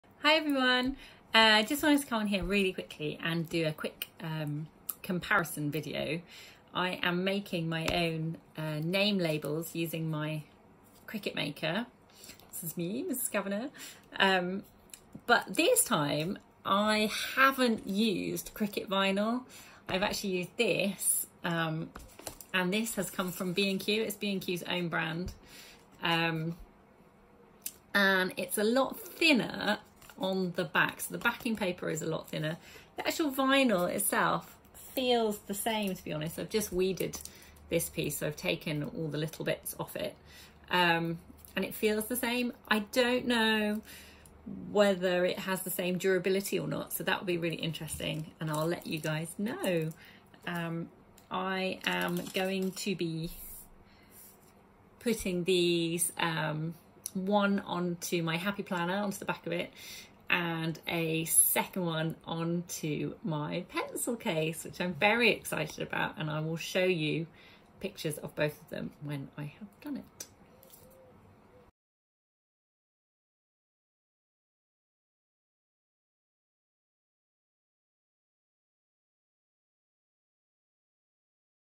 What’s the difference between D-C-Fix and Cricut vinyl.
In this video I take a look at using:

D-C-Fix Gloss Gold effect Self-adhesive film (L)150cm (W)45cm

rather than cricket branded vinyl

Cricut Gold Permanent Glossy Vinyl (L)122cm (W)30.5cm

Cricut Maker

Happy Planner